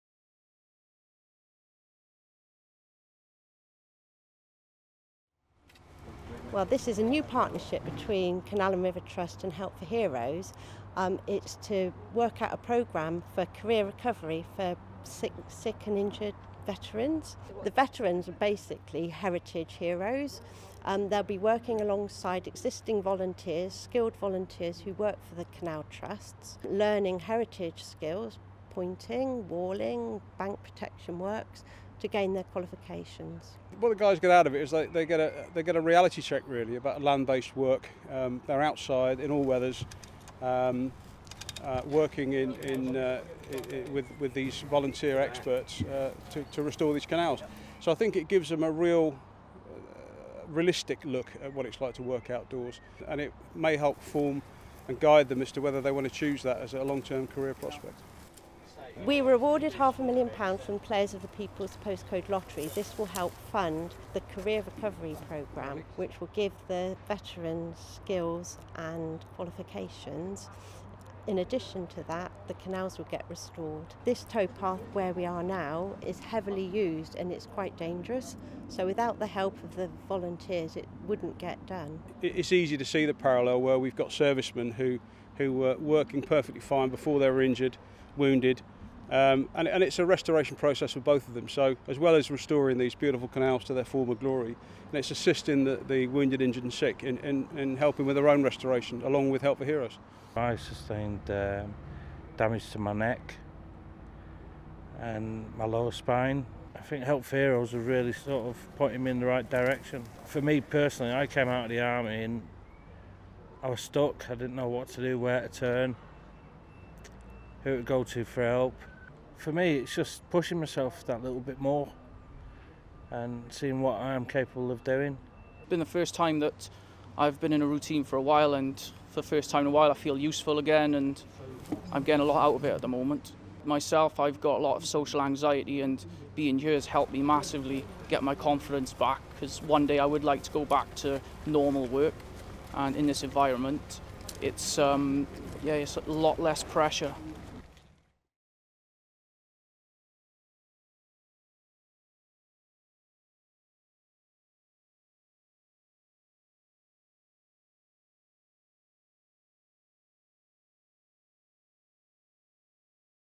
Heritage Heroes Project
Canal & River Trust and Help for Heroes have come together to give injured servicemen and women the opportunity to re-build their lives, gain qualifications and learn new skills by transforming and restoring historic canals.